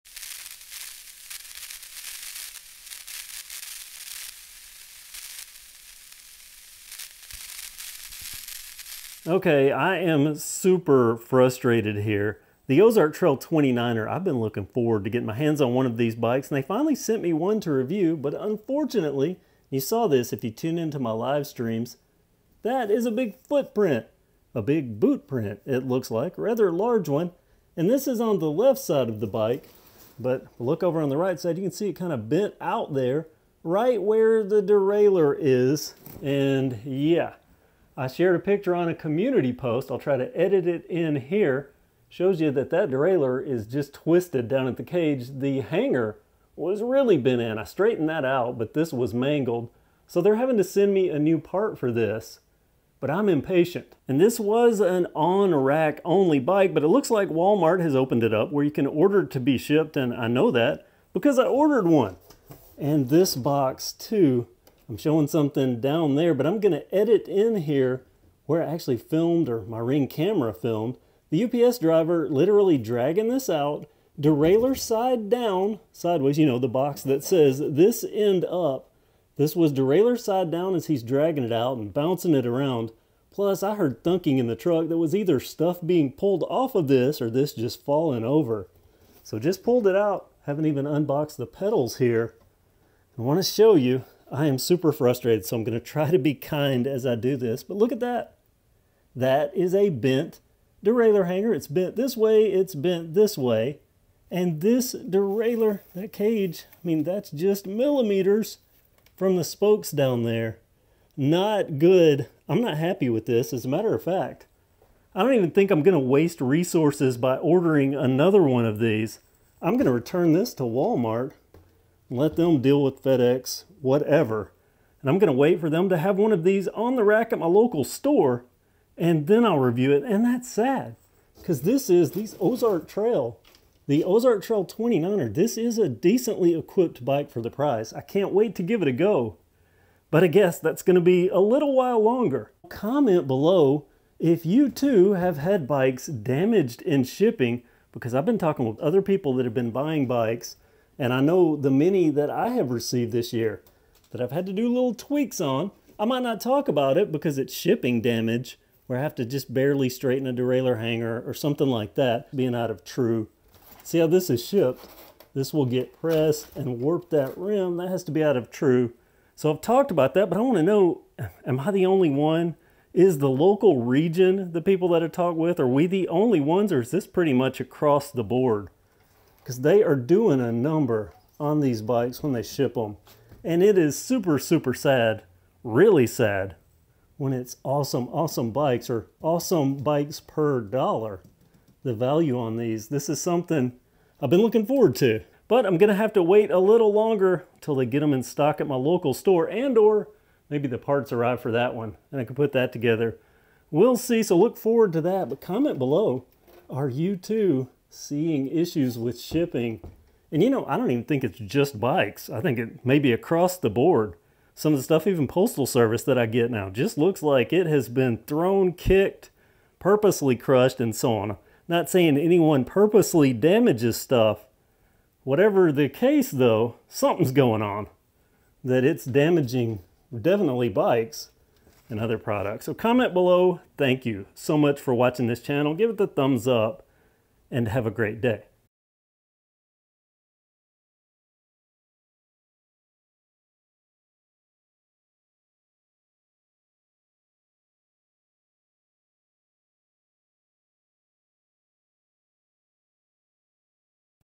DAMAGED in SHIPPING! Another Ozark Trail 29 Ridge MTB arrives damaged - Review coming EVENTUALLY!
The Ozark Trail 29-inch Ridge Mountain Bike - a bike I have been desperate to get ahold of - arrived damaged during FedEx shipping. Previously, these were only available in-store (and not in any stores around me,) so I was happy to see one available for shipping after the review unit I was sent arrived badly damaged. Sadly, this one too arrives with a bent hanger and a slightly tweaked (albeit less so than the first) derailleur cage.

I spent $435.xx on this. It's frustrating to have something new arrive already in need of repairs. SO. I have made the decision to return this and wait out finding one on the rack so I can inspect it BEFORE purchase - unless parts to repair the first unit arrive before then.

Ozark Trail 29er Ridge MTB

HP printer owner? Use this code for 1-FREE month of Instant Ink for compatible printers